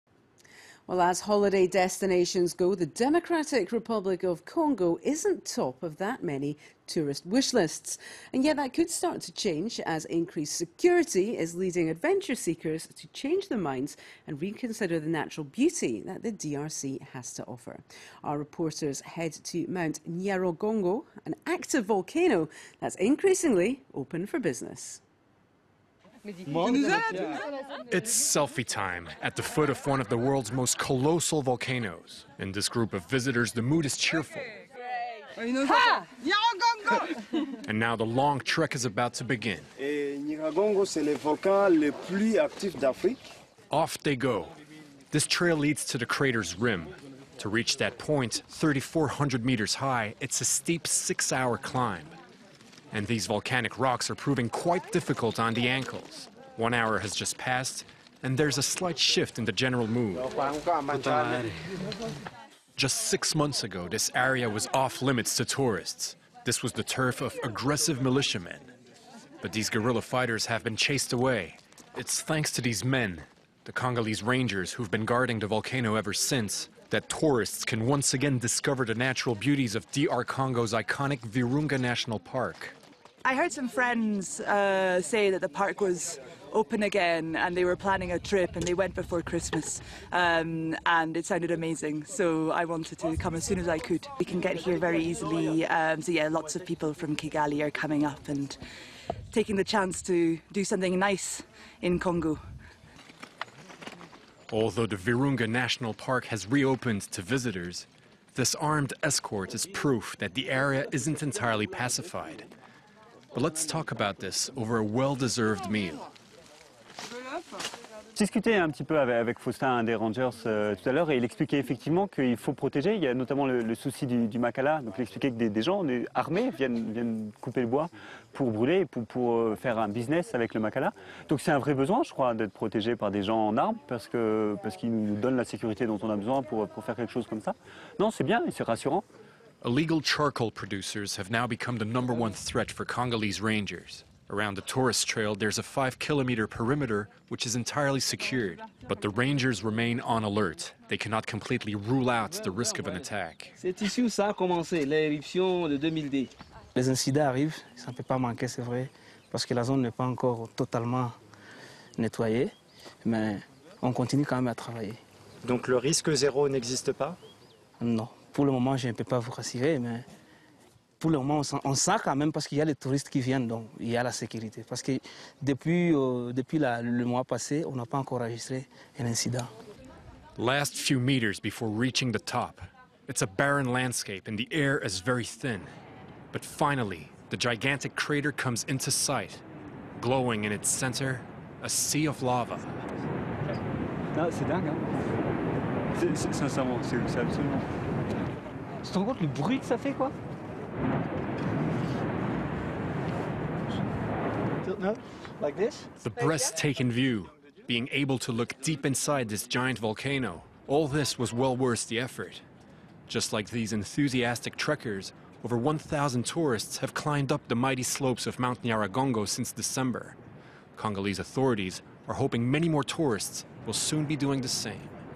DR Congo: Tourists return to Mount Nyiaragongo volcano
FOCUS : As holiday destinations go, the Democratic Republic of Congo isn't currently top of that many tourist wish lists. But increased security is leading adventure seekers to change their minds and reconsider the natural beauty the DRC has to offer.